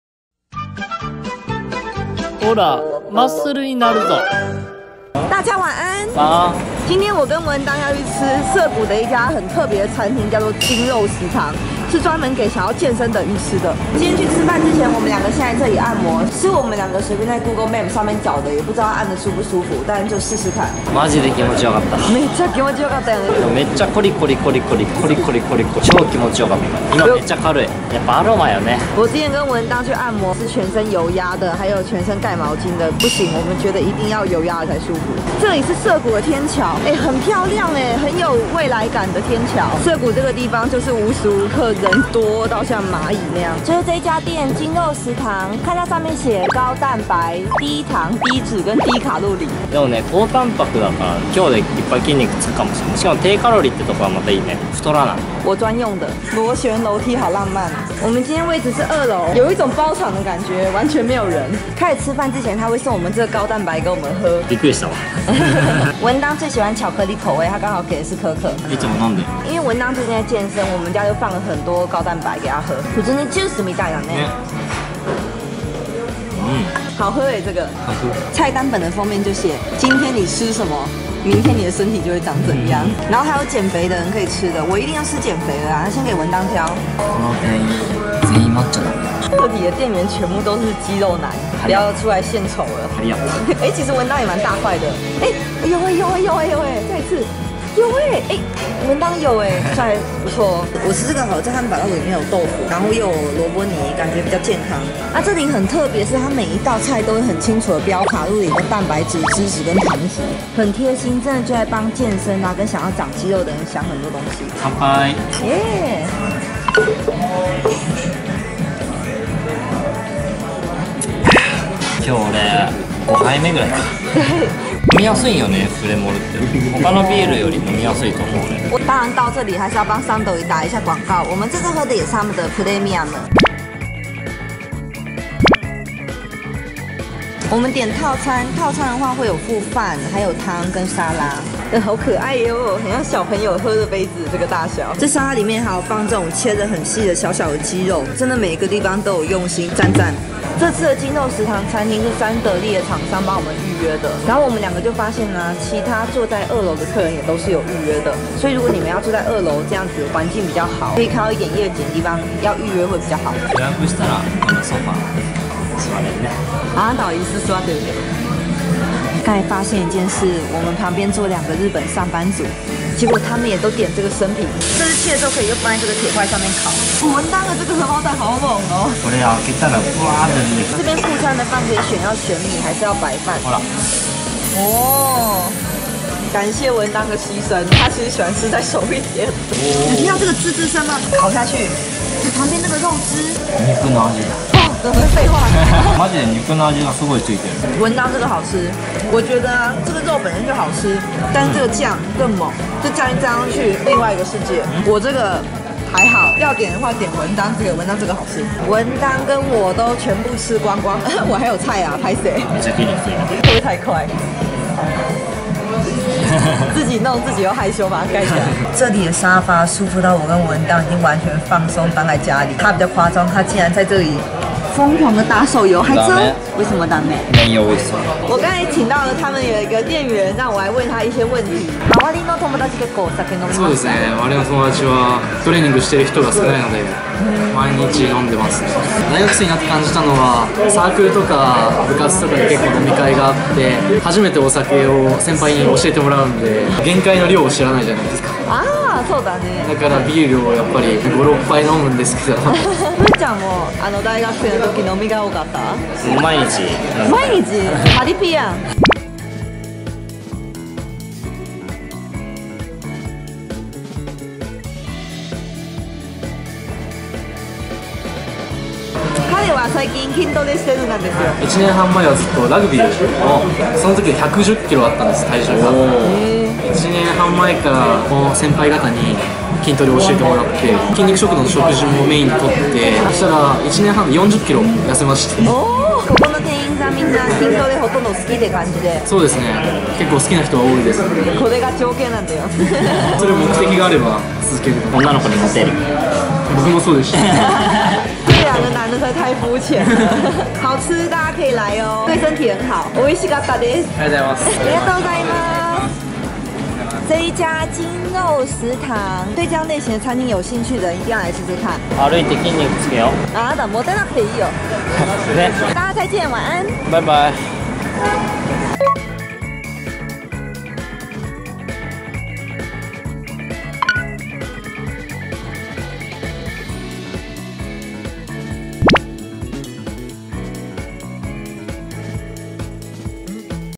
【東京人氣餐廳筋肉食堂】終於去到了！店員每個都是肌肉男～
一直都有聽過這家餐廳，終於有機會去了～
附上筋肉食堂的官網，有很多分店，有興趣的大家可以看看哪家比較方便就去哪～

然後要點的話我推薦文當點的
牛赤身肉のハンバーグステーキ

我比較不推薦我的是因為裡面有軟骨，但我本身比較不喜歡軟骨
喜歡軟骨的人可能也可以點點看～
我點的是這個 鶏ムネ肉と軟骨の豆腐入りおろしポン酢ハンバーグ
►攝影機
・SONY HDR-CX680
・DJI OSMO POCKET